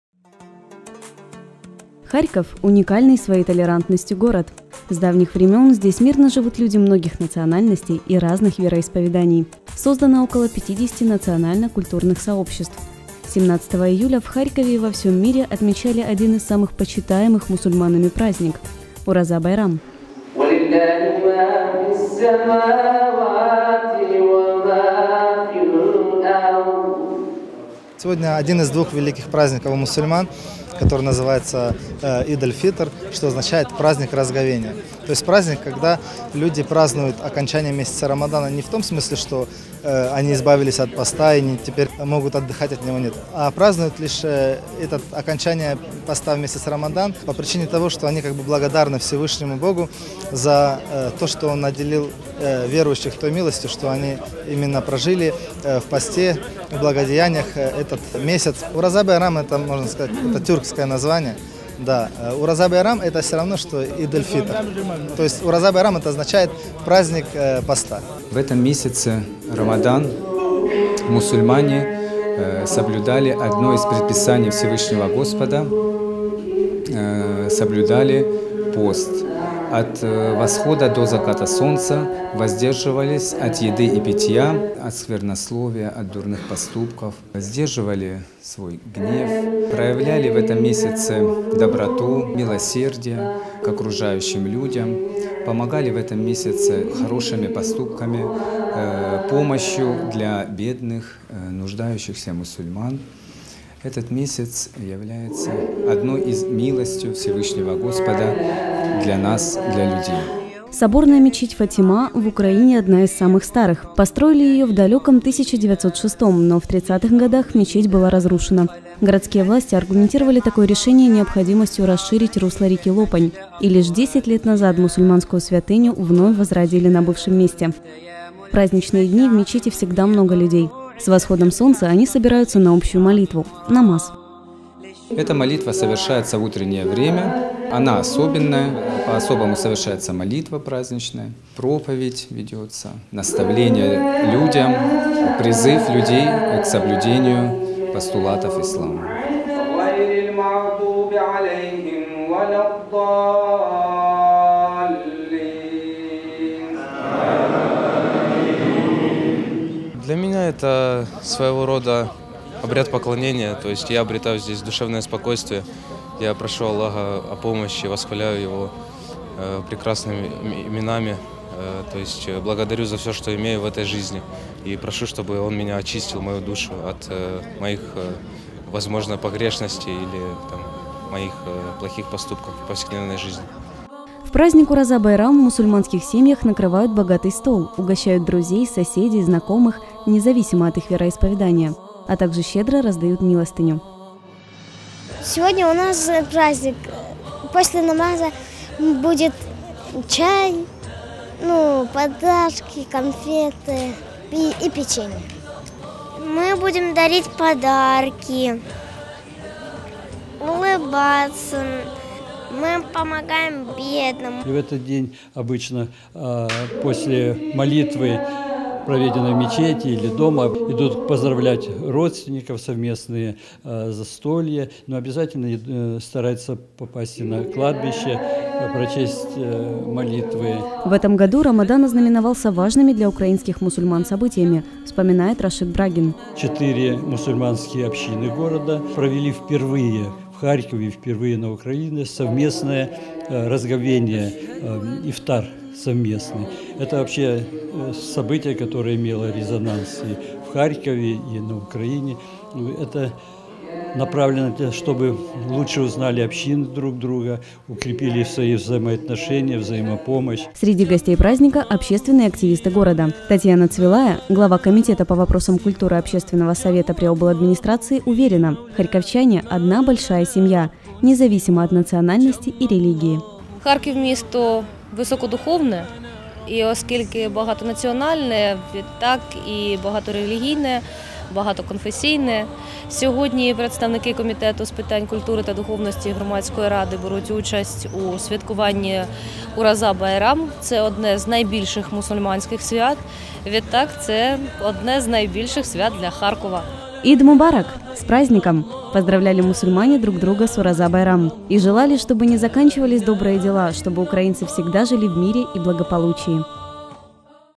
Ураза Байрам праздновали в Харькове
Партнер показа 17 июля мусульмане всего мира праздновали Ураза Байрам- один из самых больших мусульманских праздников. В Харьковской соборной мечети в этот день было особенно многолюдно.  С восходом солнца прихожане собрались на совместную молитв у- намаз.